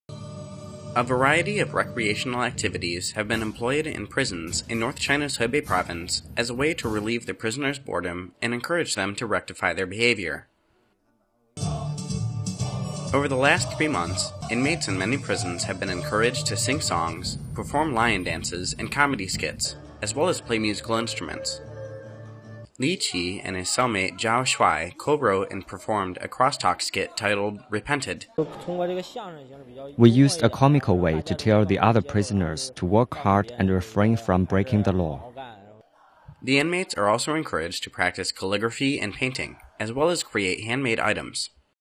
Chinese prisoners use entertainment for rehabilitation
A variety of recreational activities have been employed in prisons in north China’s Hebei province as a way to relieve the prisoners' boredom and encourage them to rectify their behavior.